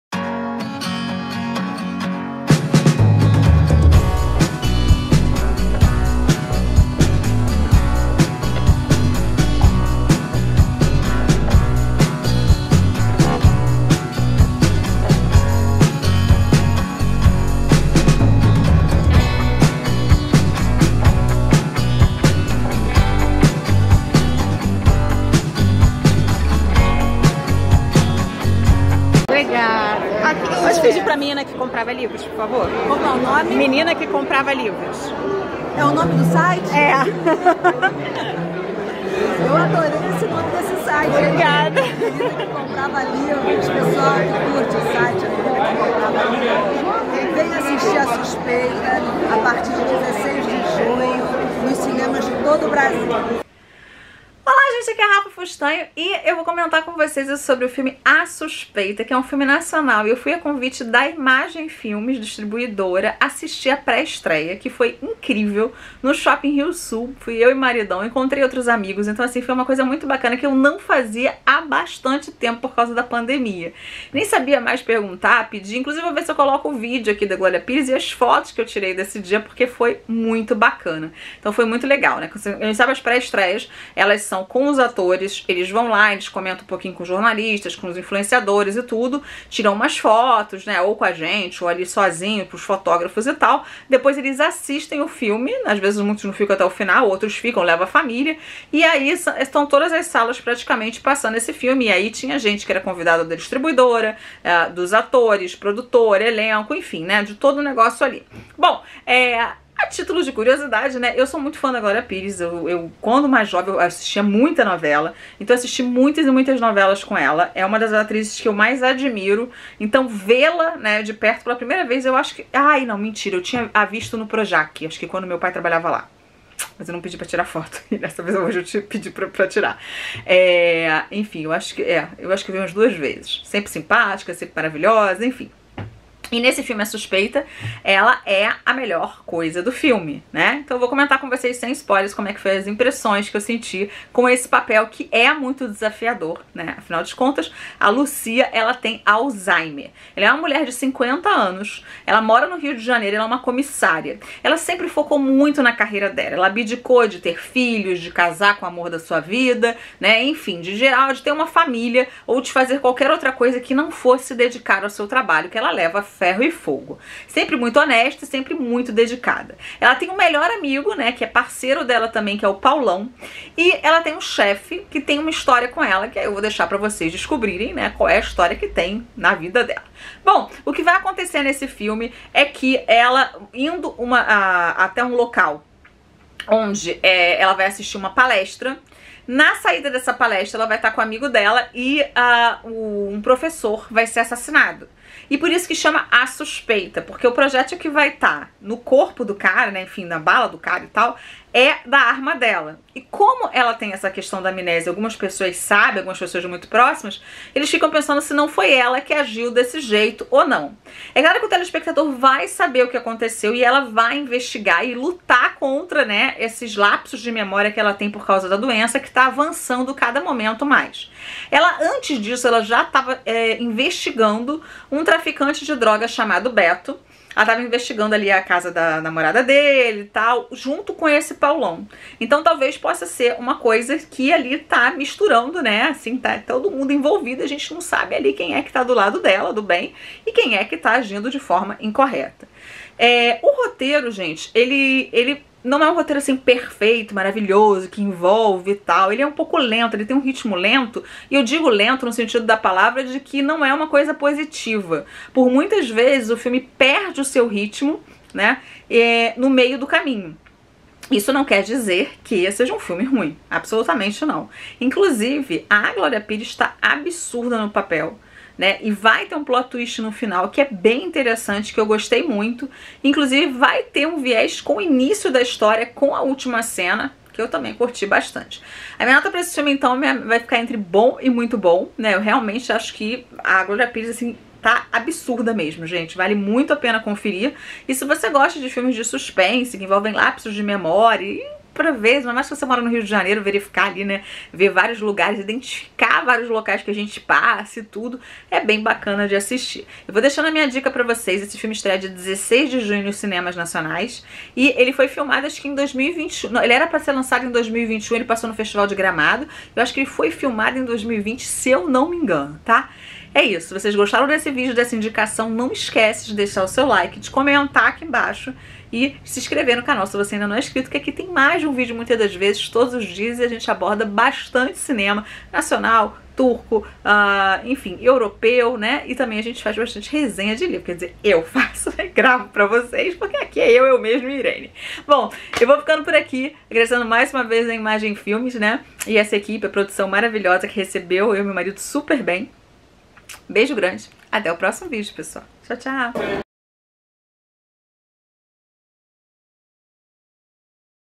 A Suspeita 🎬Pré Estreia + Comentários sobre o filme
Título Original: A Suspeita
País: Brasil
Ano: 2021
Lançamento nos cinemas em 16 de junho de 2022
2h 00min / Ação
Direção: Pedro Peregrino
Roteiro Thiago Dottori, Newton Cannito
Elenco: Glória Pires, Charles Fricks, Gustavo Machado

Agradecimentos à Imagem Filmes pelo convite da pré estreia

💙Minha página de Autora na Amazon